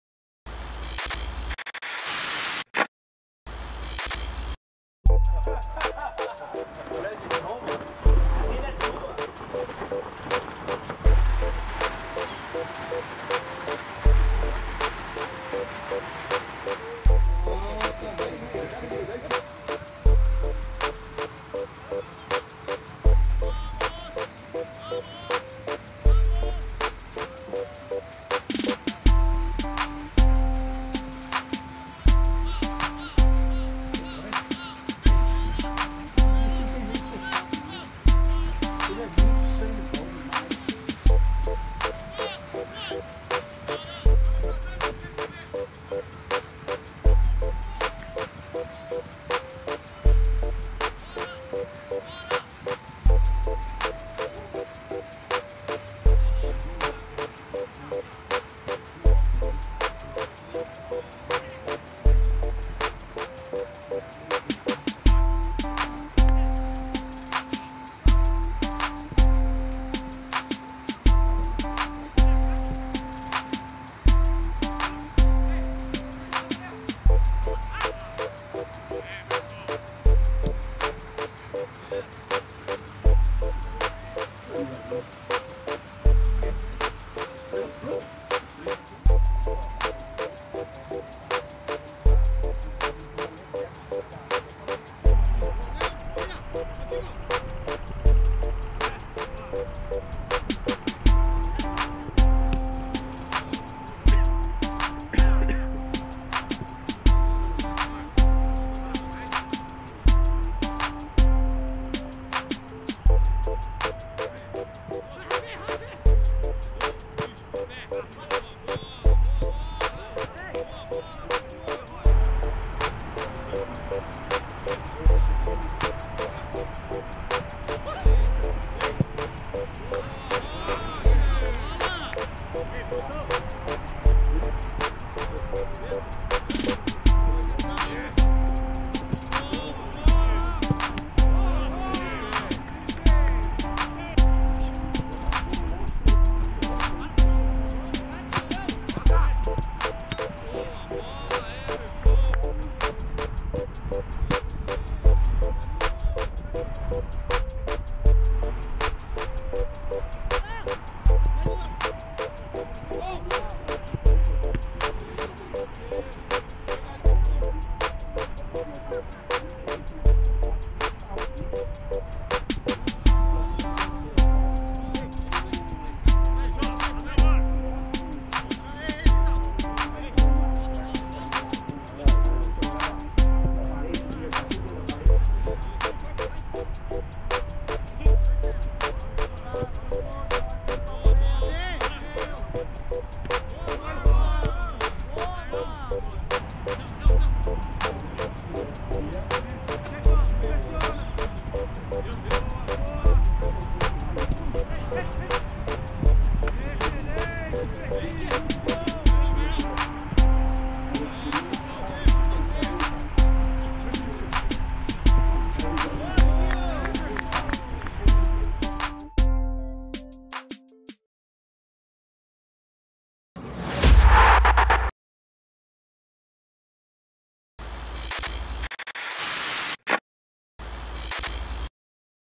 Treino tático e campo reduzido
Antes de embarcar para o Chile, o técnico Osmar Loss comandou o último treino da equipe no CT Dr. Joaquim Grava! Confira como foi o treinamento pelas lentes da Corinthians TV.